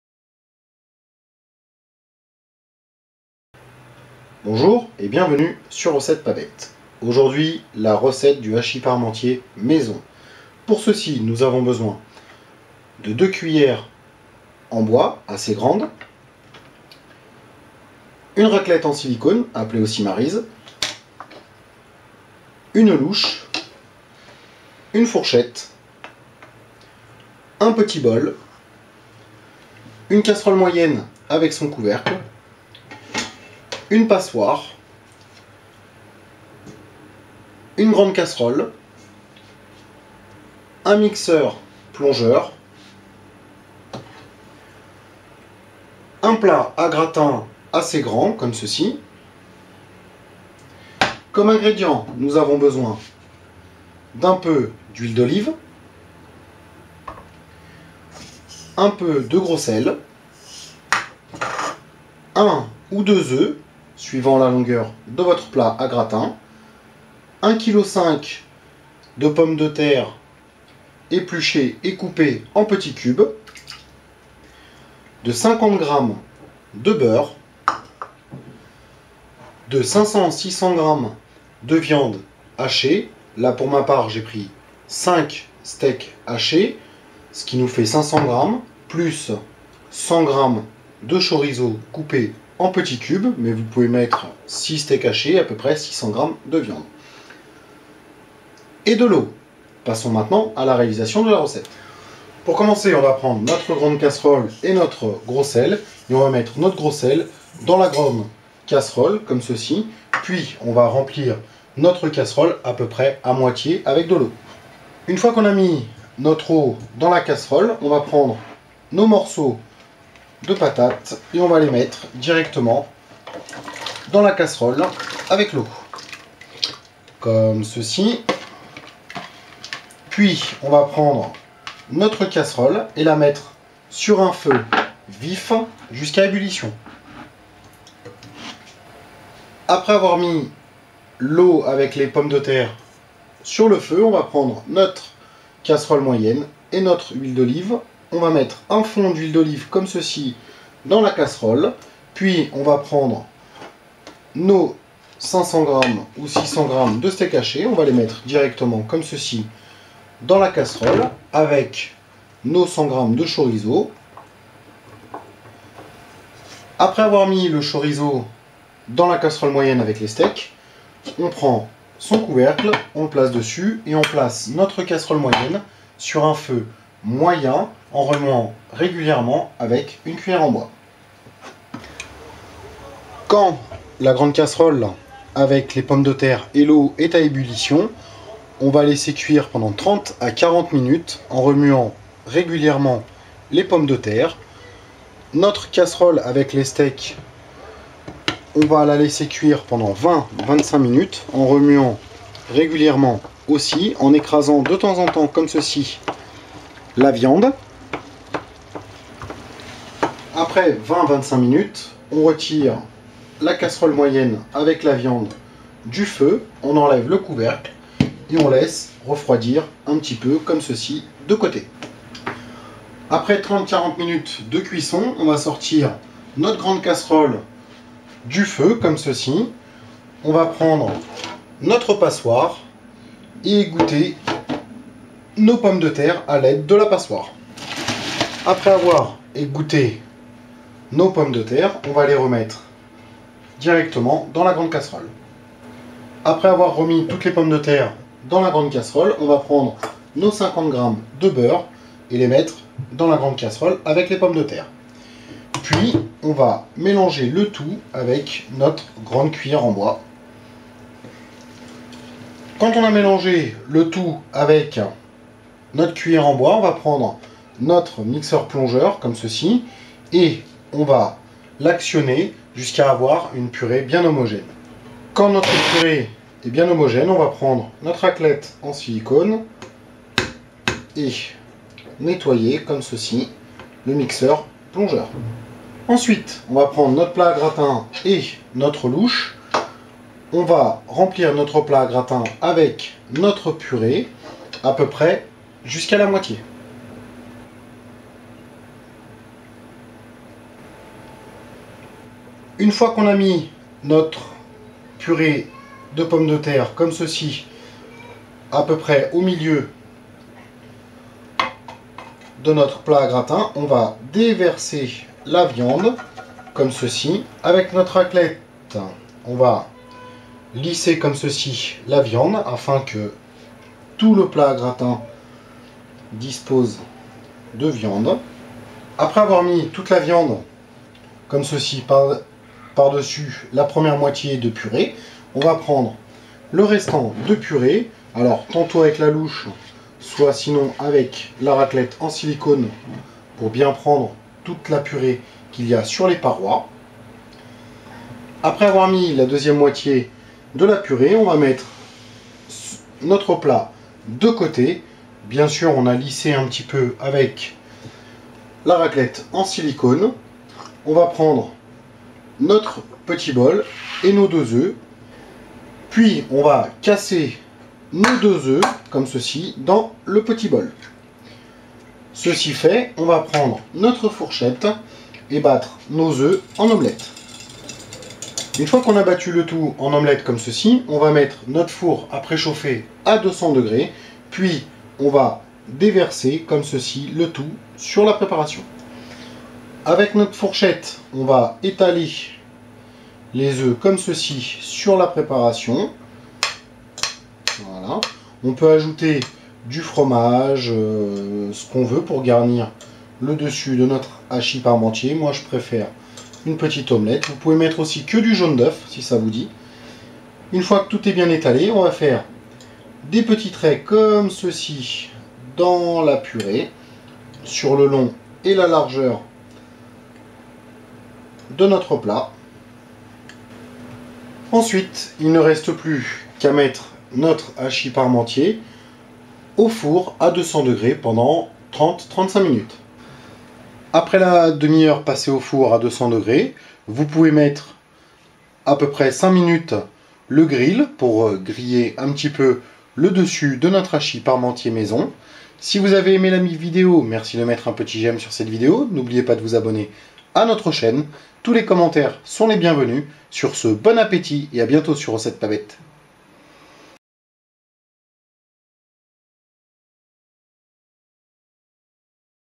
hachi-parmentier crémeux (recette facile) HD
La recette en vidéo du hachi-parmentier crémeux par recettes-pas-bête, expliquer pas à pas
Retrouvez la liste des ingrédients plus bas et le lien de la recette complète.
Liste des ingrédients :
un peu d'huile d'olive
du gros sel
1-2 œufs
1.5 kg de pommes de terre
50g de beurre
500-600g de viande hachée
Le matériels requis :
1 Plat a gratin ovale (acheter le
1 grande casserole et son couvercle (4L de contenance
1 casserole et son couvercle (2l de contenance)
1 grand bol (2L de contenance)
2 cuillère en bois
1 bol
1 cuillère à café et 1 cuillère à soupe
1 éplucheur économe
1 louche
1 passoire
1 raclette en silicone